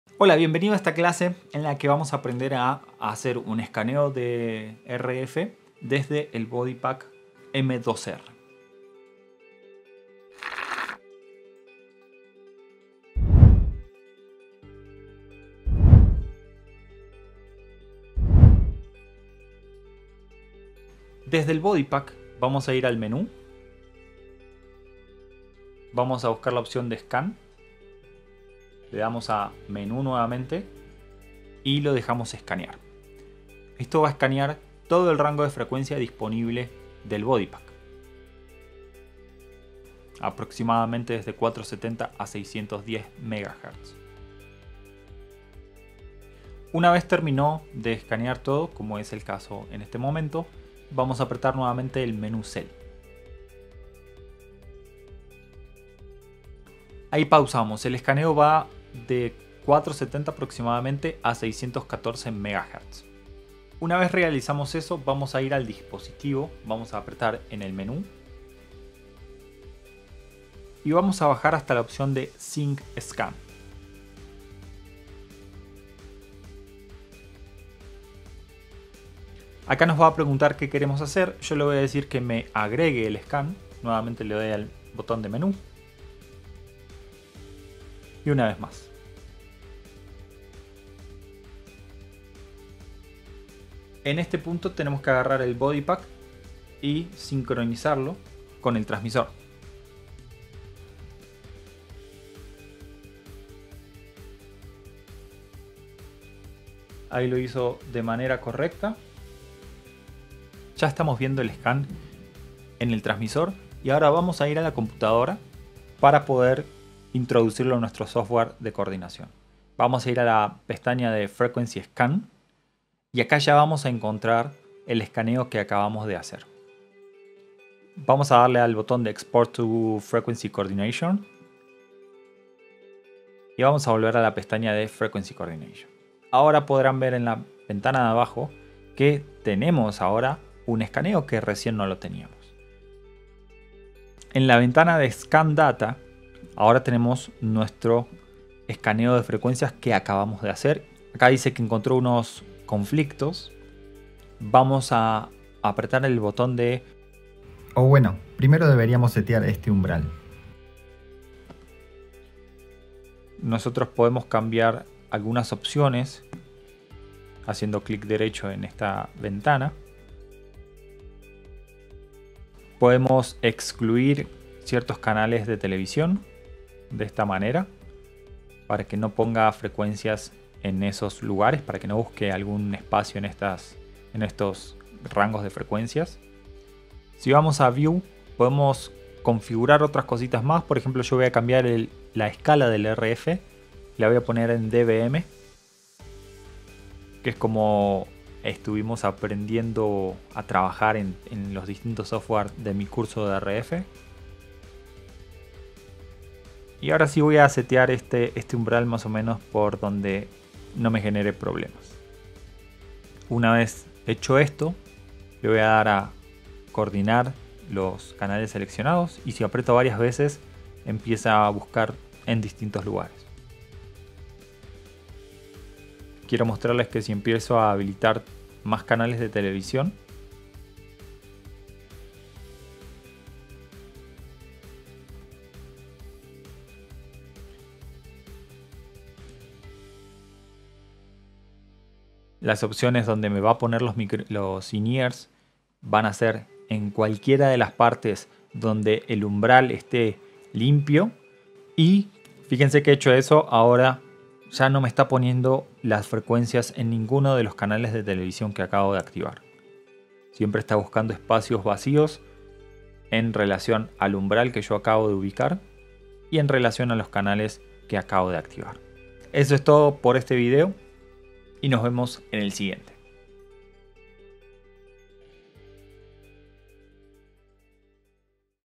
3-Escanear el espectro desde el receptor M2R de Lectrosonics
En este video aprenderás a hacer un escaneo de frecuencia con el receptor (bodypack) del M2T de Lectrosonics. Esto te servirá para no depender de algún analizador de espectro externo.